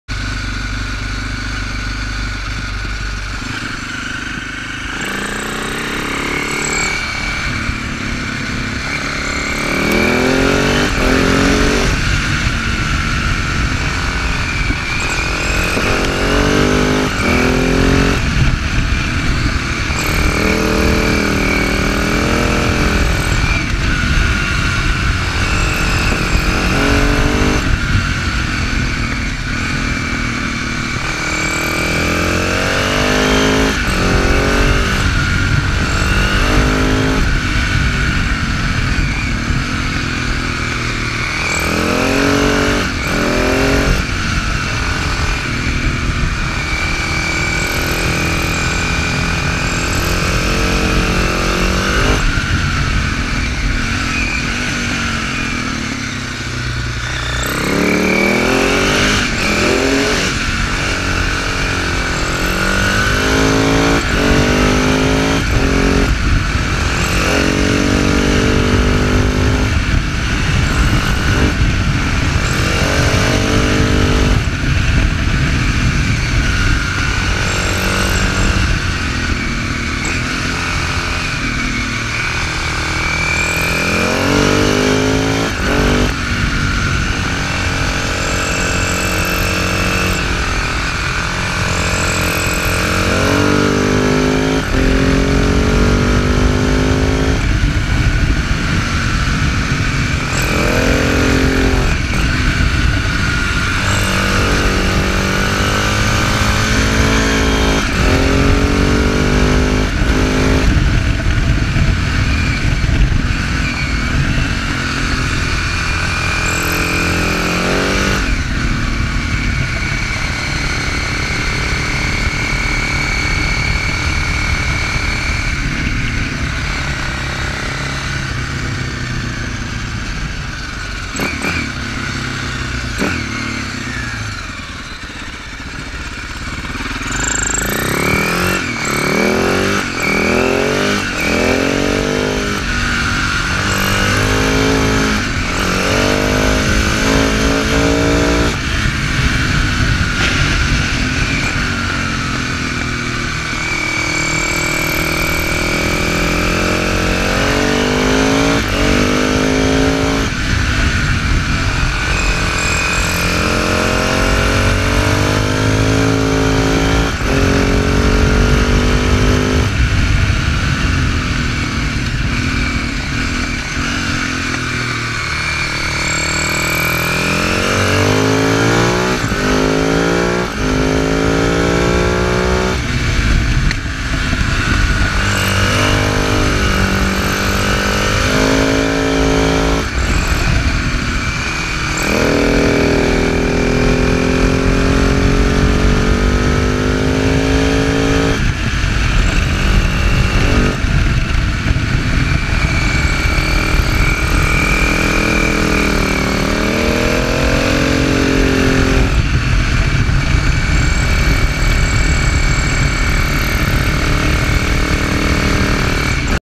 A short trip from Brienz to Alvaneu HUSABERG FS650 Supermoto
A short trip from Brienz to Alvaneu. In the swiss mountains.